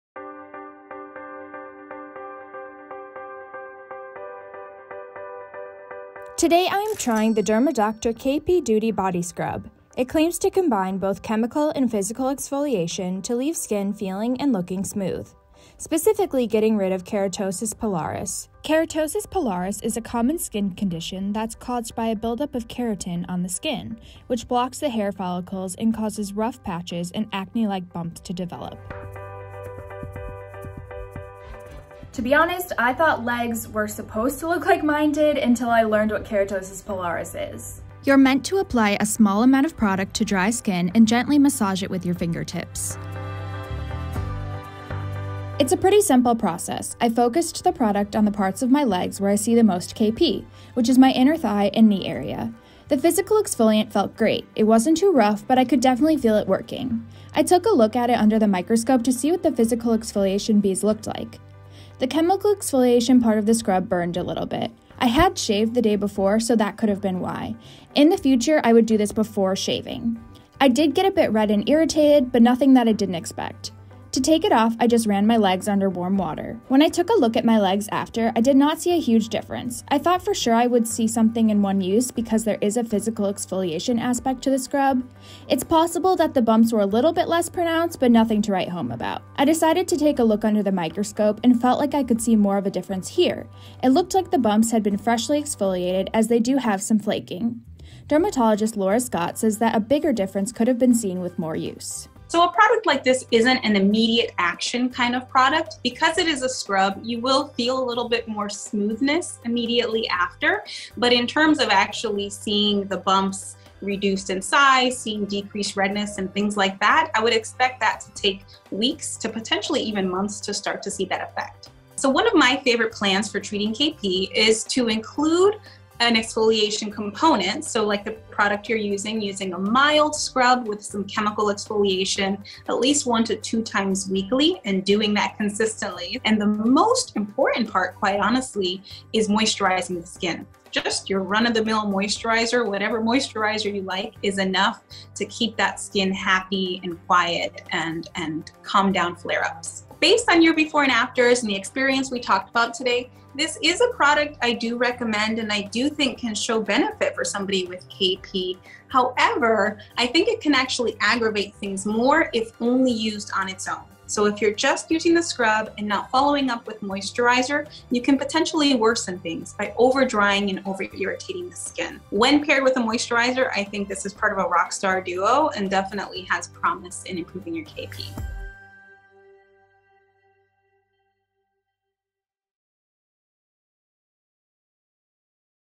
Body Scrub Uses Chemical And Physical Exfoliants To Get Rid Of Keratosis Pilaris | The Skin Fix
Keratosis pilaris, or strawberry skin, is a common skin condition that causes rough patches and small, acne-like bumps on the skin. It develops when the skin produces too much of a protein called keratin, which can block hair follicles and cause bumps to develop. Dermadoctor KP Duty Body Scrub claims to combine both chemical and physical exfoliation to leave skin feeling and looking smooth, specifically getting rid of keratosis pilaris. Insider producer Celia Skvaril tried out the KP Duty body scrub to see if it could get rid of the strawberry skin on her legs.

Check out the Dermadoctor KP Duty Body Scrub here:
Dermadoctor KP Duty Body Scrub, $32.77

Check out the microscope here:
Celestron Deluxe Handheld Digital Microscope, $72.48

MORE SKIN FIX VIDEOS:
Heated Needle Tool Claims To Remove Skin Tags And Moles At Home | The Skin Fix | Beauty Insider